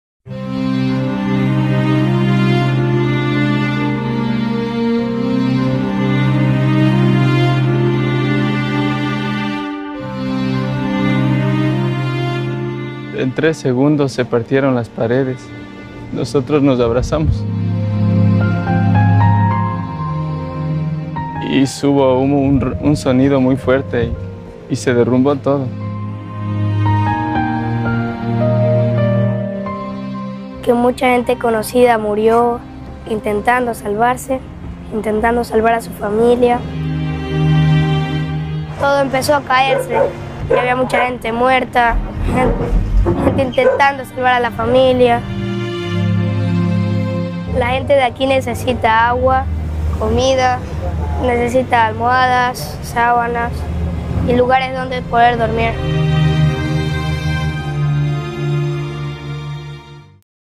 Terremoto en Ecuador miles de niños necesitan ayuda
Con tu ayuda estamos trabajando para responder a la emergencia por el sismo en Ecuador y proteger a miles de niños y sus familias que viven en las zonas afectadas